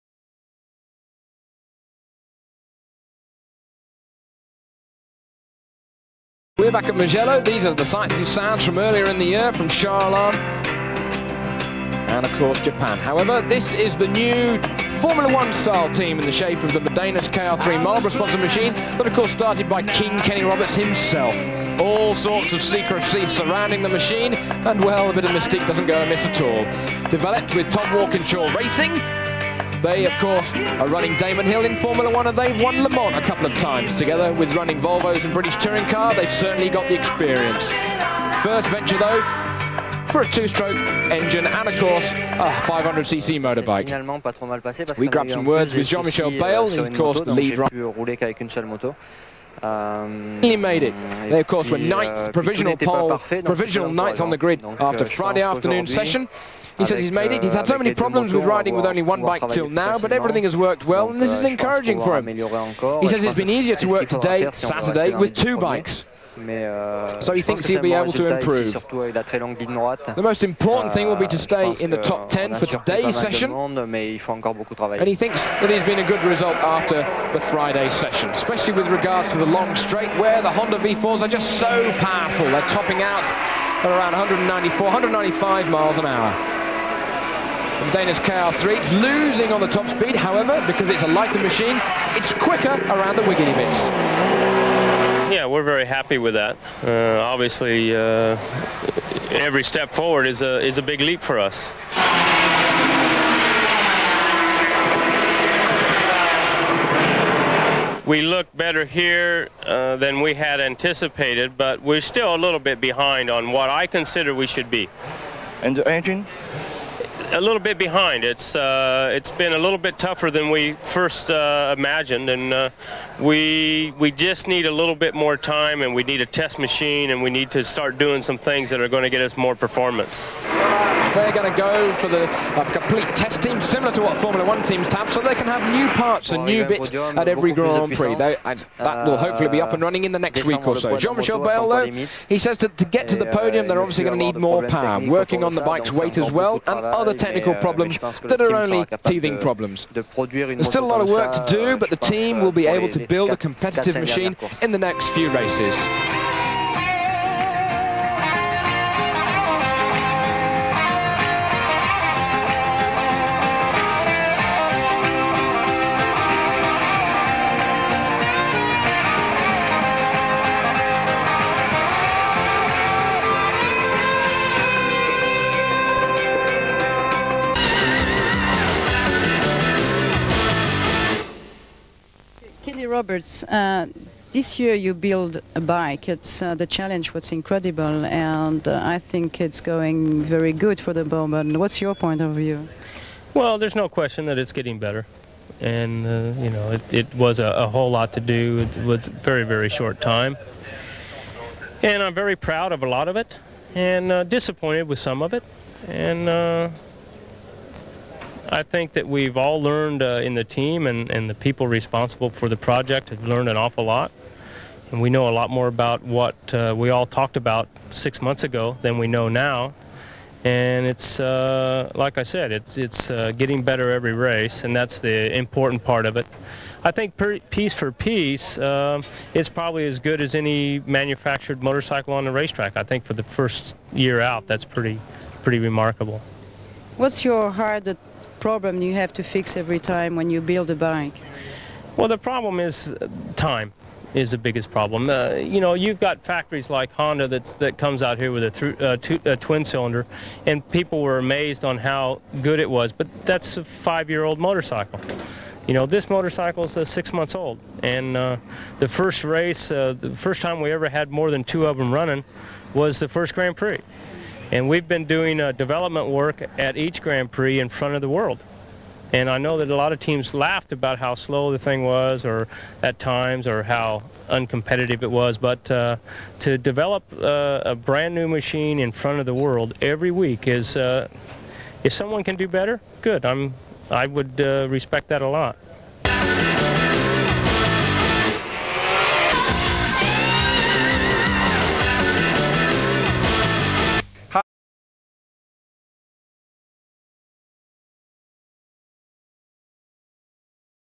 GP 97 JEAN MICHEL BAYLE TEAM MODENAS ROBERTS byraulugena
Reportajes en inglés emitidos por Eurosport durante la temporada 1997 sobre el piloto francés y el equipo Roberts de 500 de esa temporada.